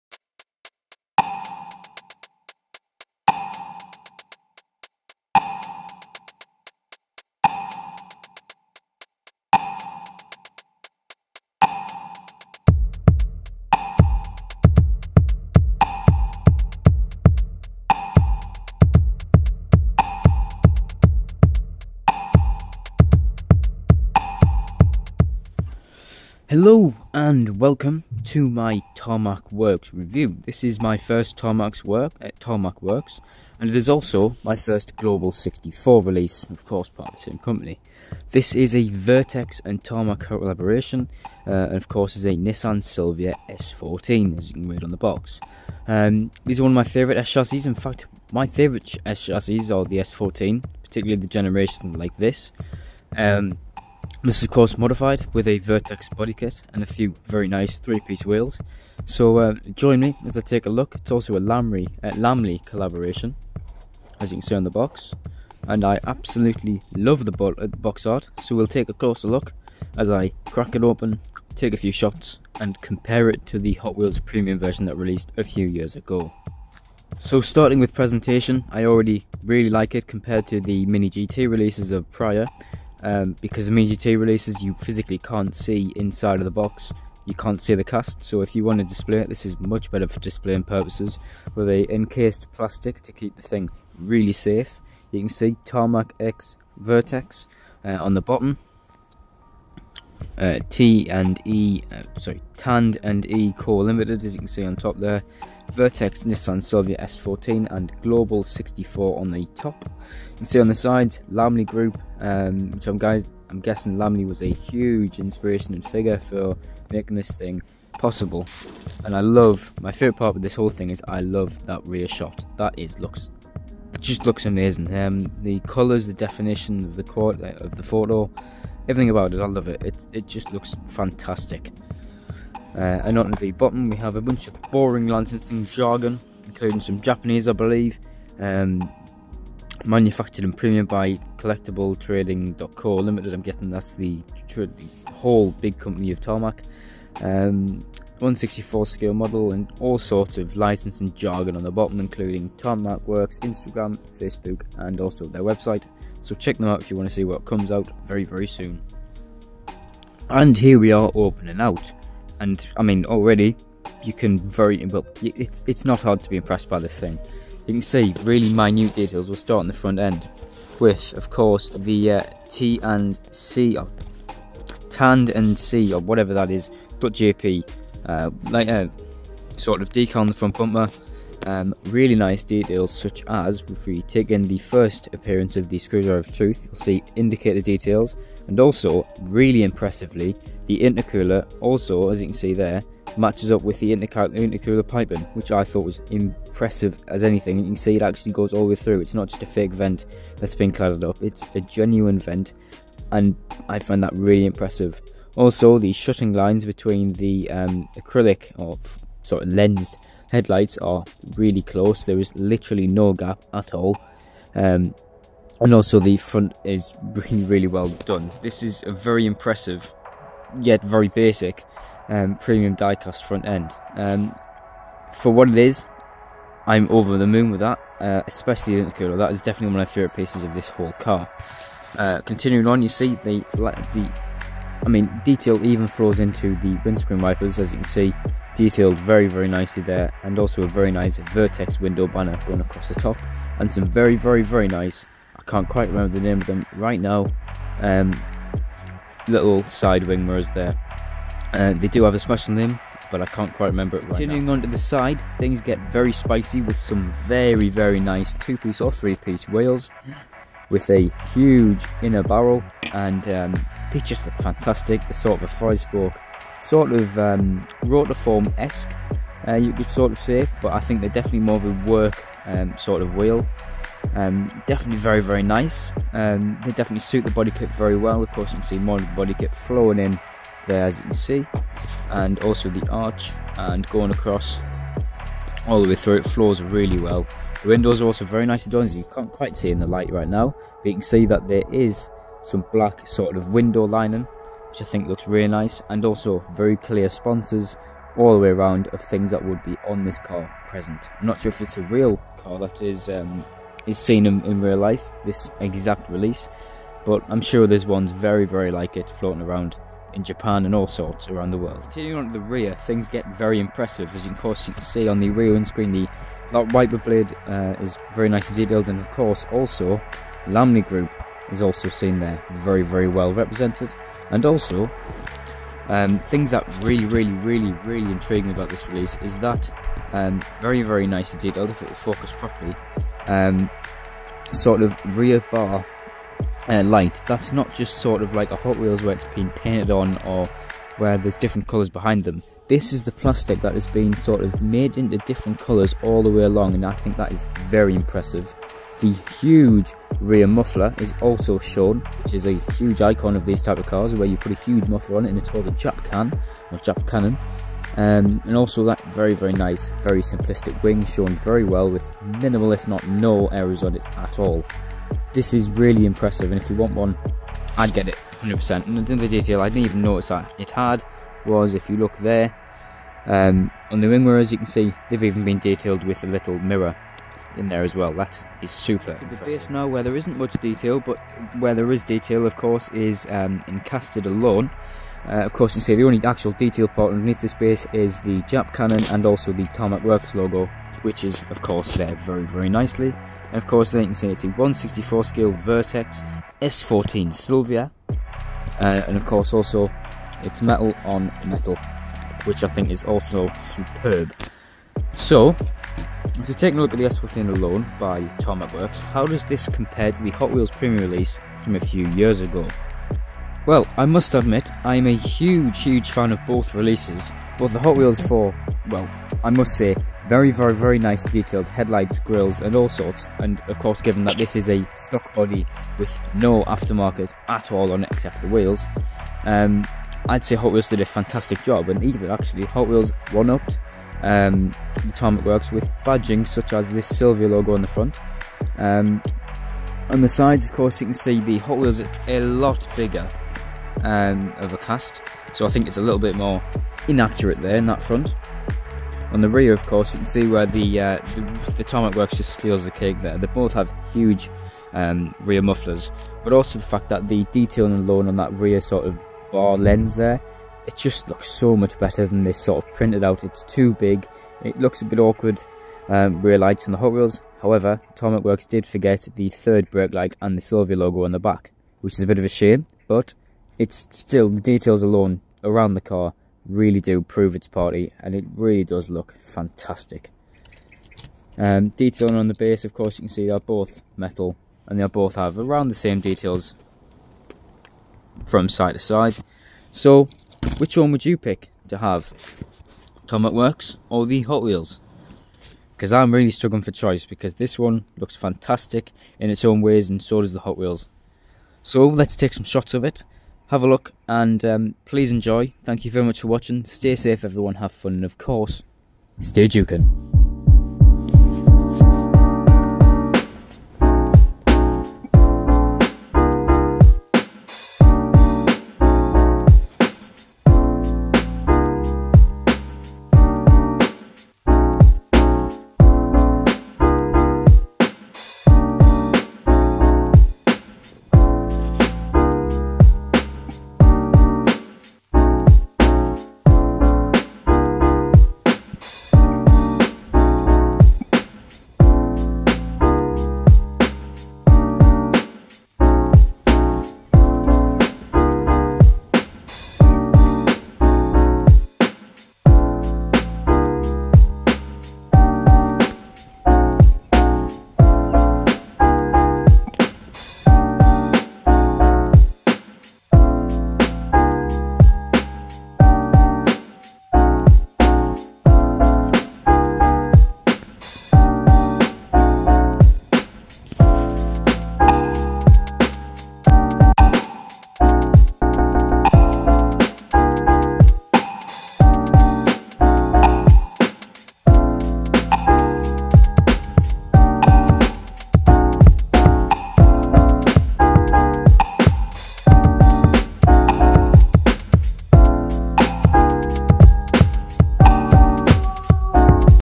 TARMAC WORKS NISSAN SILVIA S14// @lamleygroup GLOBAL 64 RELEASE
What a fantastic start to my tarmac works collection, a brilliant vertex nissan Silvia s14!
with details as small as a spec of dust, and a cool factor larger than the sun, it's safe to say I'm hugely impressed with this release!

I love the clean white and silver colour scheme, it works incredibly well with the smooth yet aggressive vertex body kit!

stay safe
stay juke'ing!!